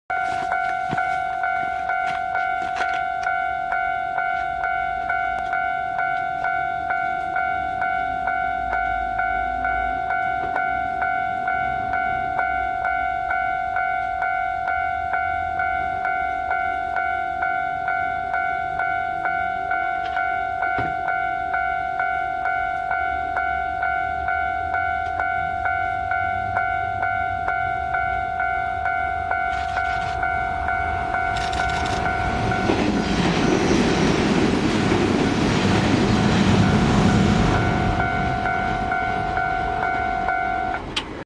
嵯峨野線を行く381系特急きのさき
6月1日から走り出した381系による特急きのさき

山陰本線亀岡～馬堀

２０１２・６・７